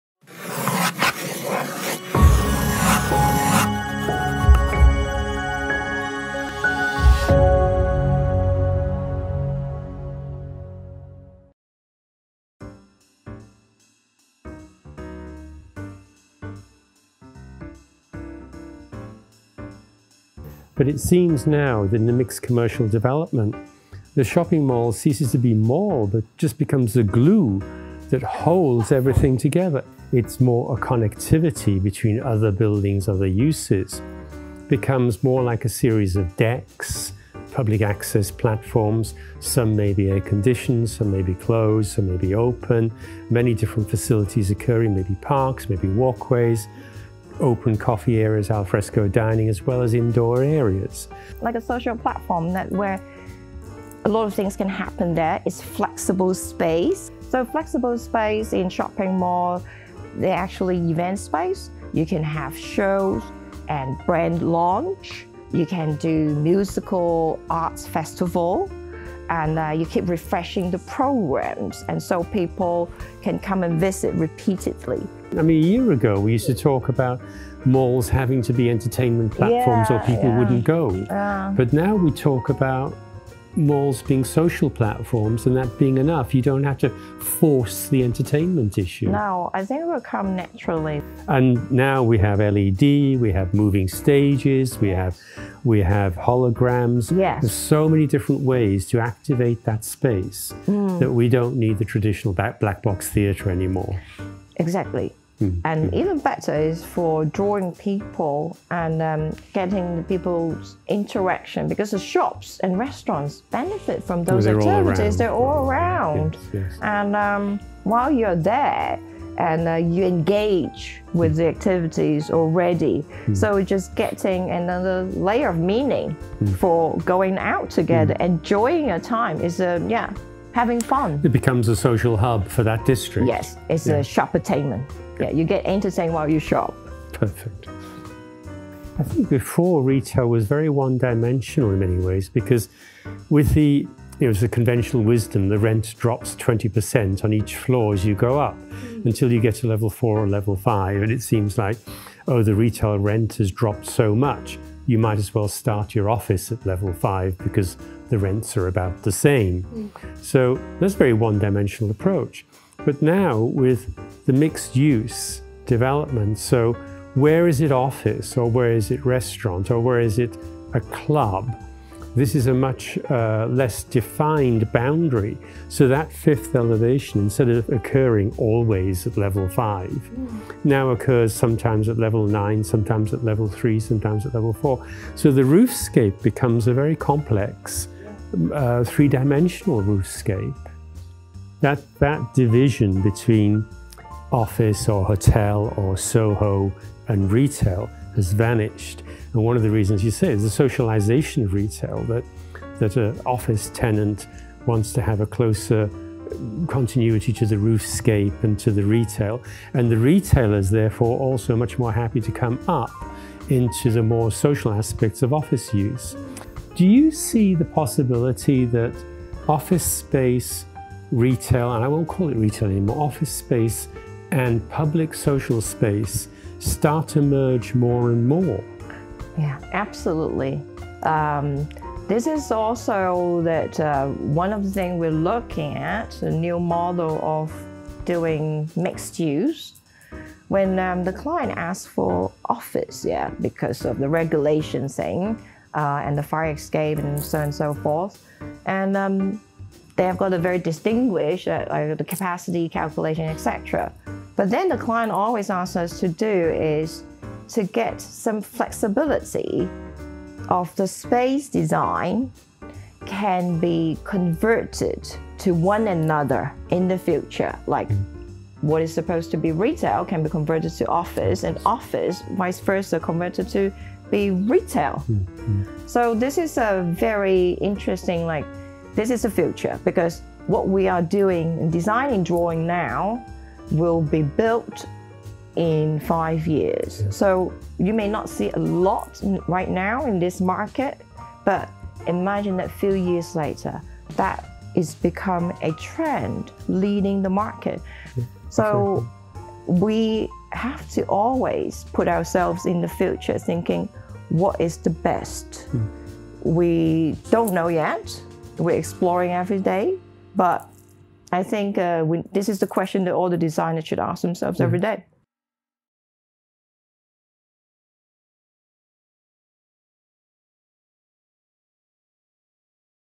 Retail Trends in a Post-Pandemic Era – Part 3 – Retail Malls As Common Public Space
Coming to the end of our retail trends series, Christine Lam, our Global Design Principal and Aedas Chairman Keith Griffiths, exchanged ideas on the changing operating model of mixed-use development. Shopping malls are turning into social platforms where a host of programs are continuously refreshed in flexible event spaces; there is increasing demand in public social places that roofscapes become an attraction and a necessary feature.  Find out why is it integral for architects to adopt forward-looking mindsets and future-proof designs now.